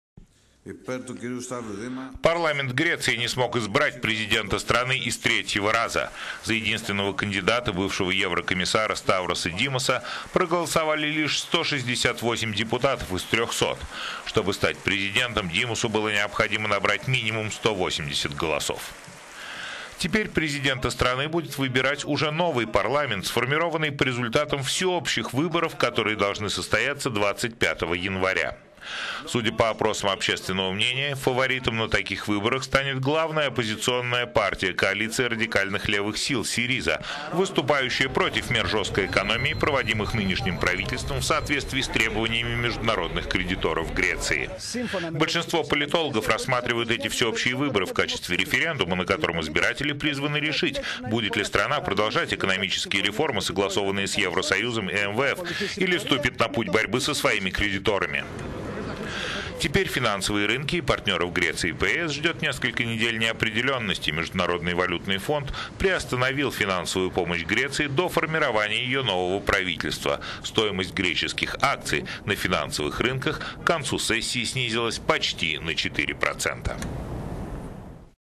Греция: президента будет выбирать новый парламент
Парламент Греции не смог избрать президента страны и с третьего раза. За единственного кандидата - бывшего еврокомиссара Ставроса Димаса проголосовали лишь 168 депутатов из 300. Чтобы стать президентом Димасу было необходимо набрать минимум 180 голосов.

Теперь президента страны будет выбирать уже новый парламент, сформированный по результатам досрочных всеобщих выборов, которые должны состояться 25 января.

Судя по опросам общественного мнения, фаворитом на этих выборах станет главная оппоз…